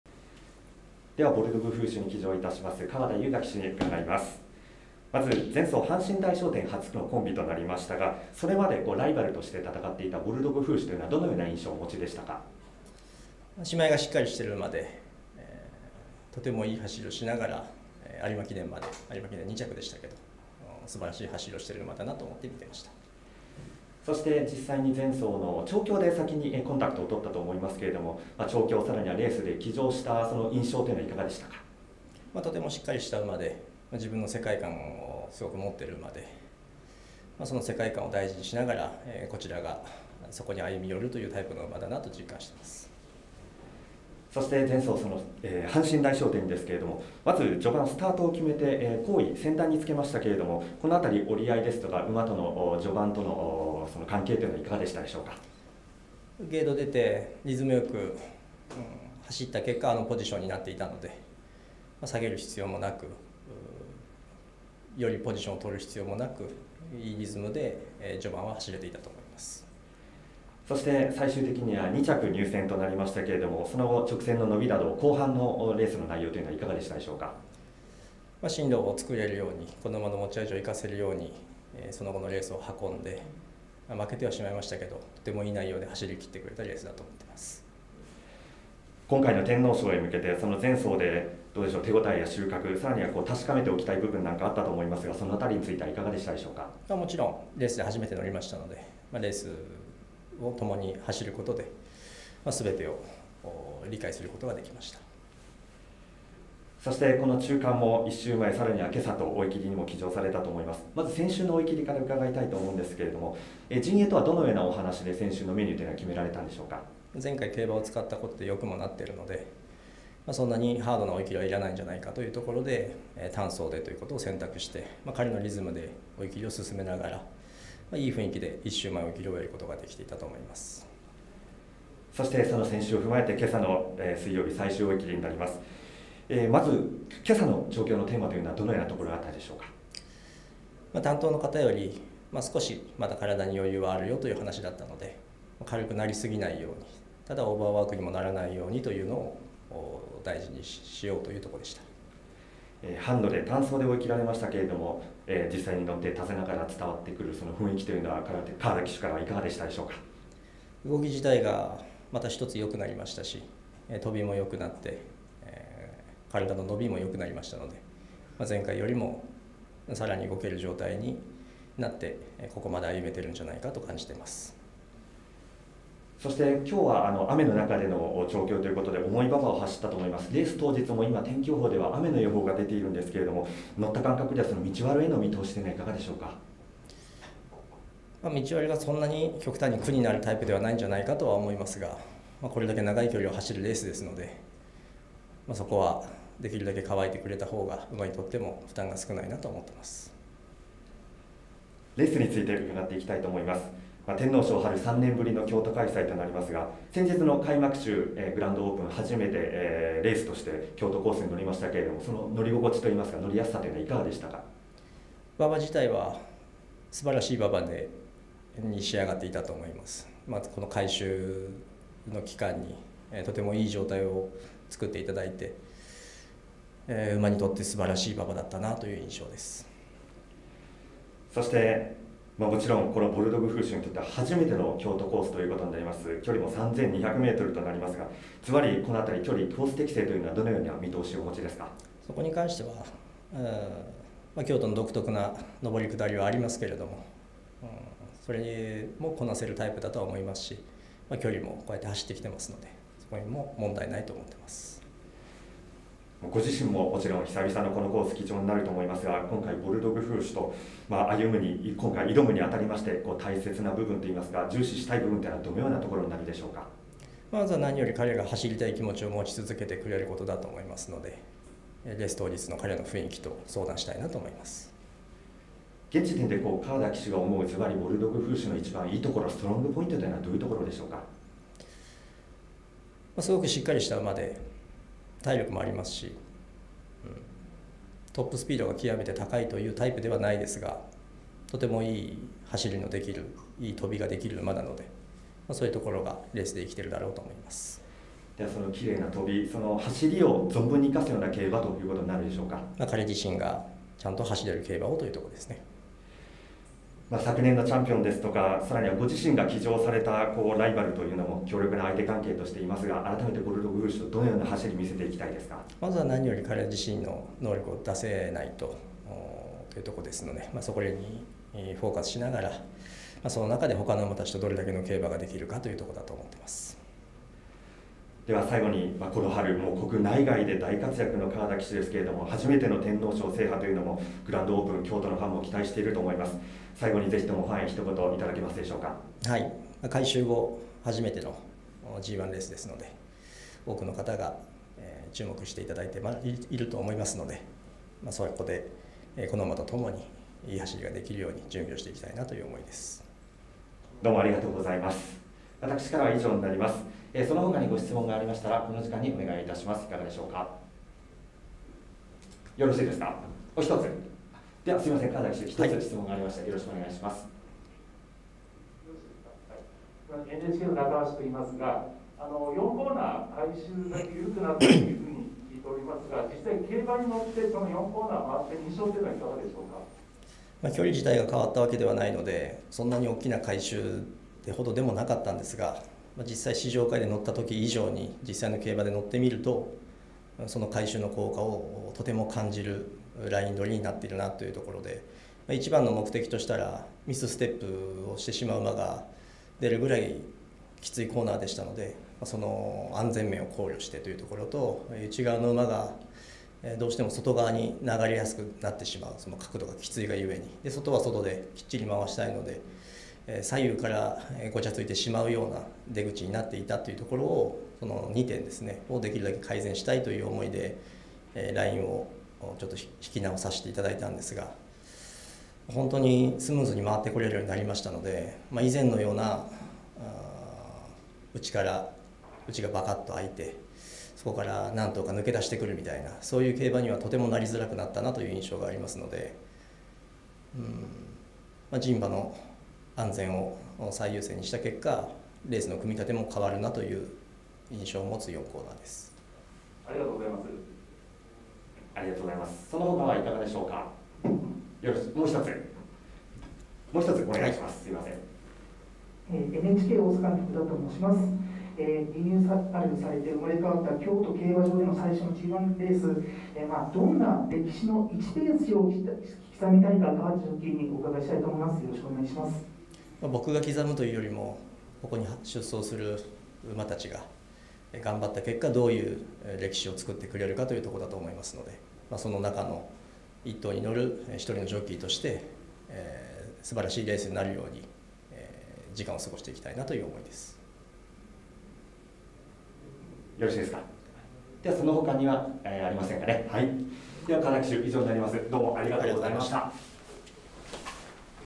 天皇賞（春）共同会見 ボルドグフーシュ・川田将雅騎手（2023年4月26日）
新世代の競馬予想ツール「Deep」とは？